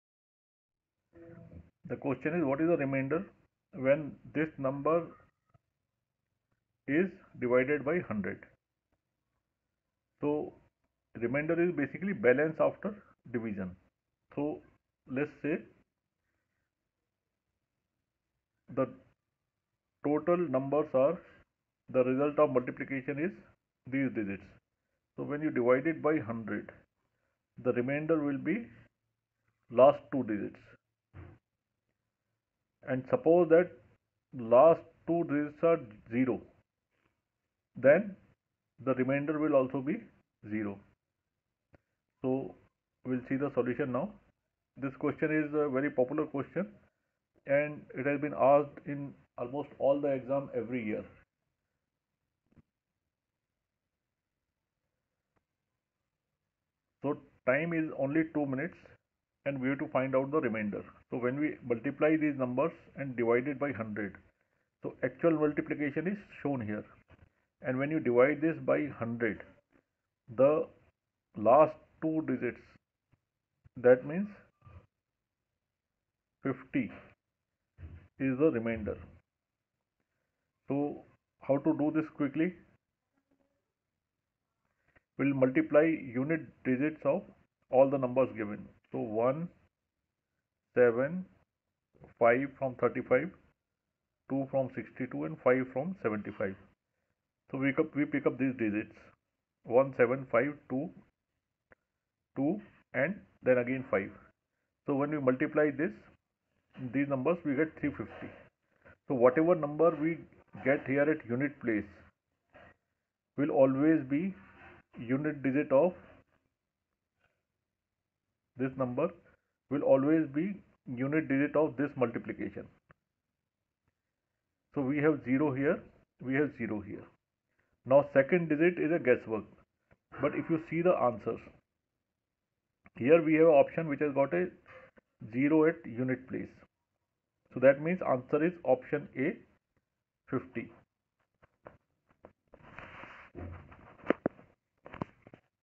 CSAT 2011 IAS prelims/What is the remainder when 51 x 27 x 35 × 62 × 75 is divided by 100 ?
CSAT 2020 IAS prelims/What is the remainder when 51 x 27 x 35 × 62 × 75 is divided by 100 ?